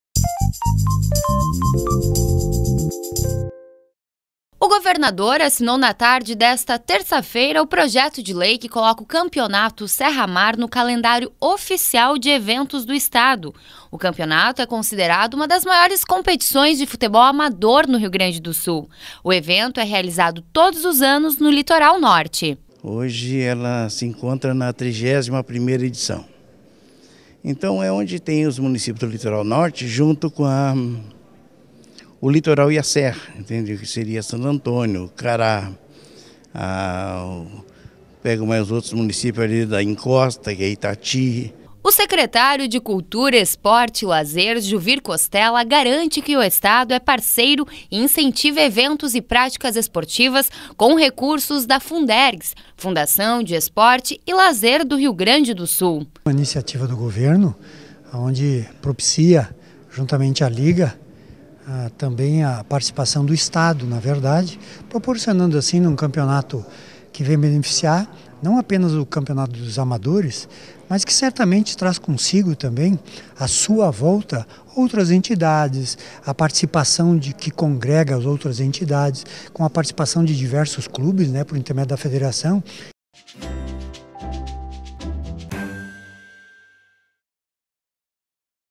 Campeonato Serramar entra no calendário de eventos do Estado
O governador José Ivo Sartori assinou, na tarde desta terça-feira (23), o Projeto de Lei que inclui o Campeonato Serramar no calendário de eventos do Estado.